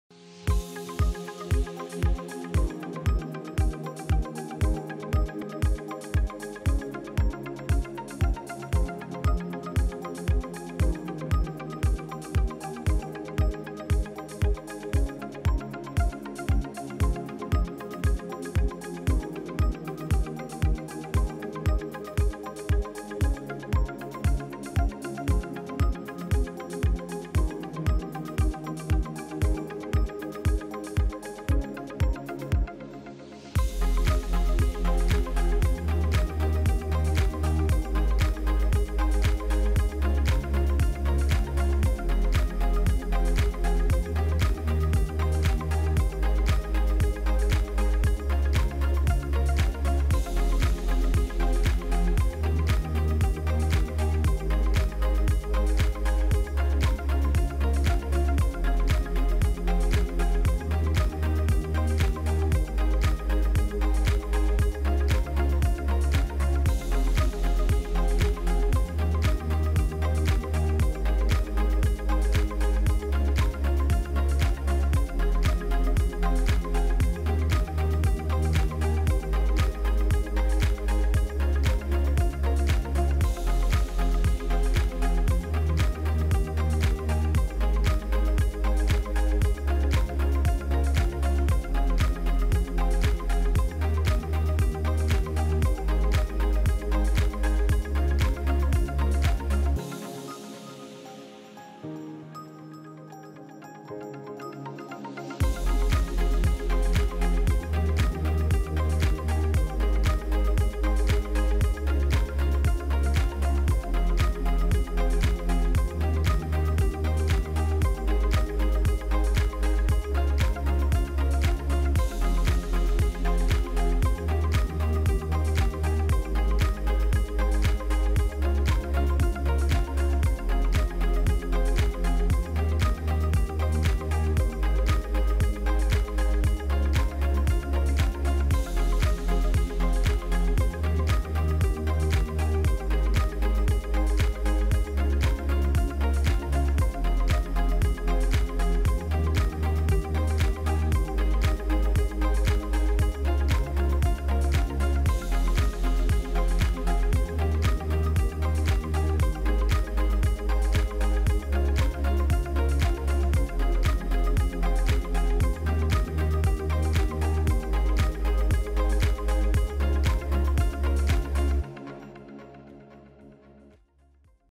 PowerFlex 755TM Drive System animation â€“ Portuguese
This animation showcases the hardware of the PowerFlex 755TM drive and represents three frame sizes (8, 10 & 12). Use this animation to demonstrate the advantages of our modularity, built-in components and flexibility of a common bus system.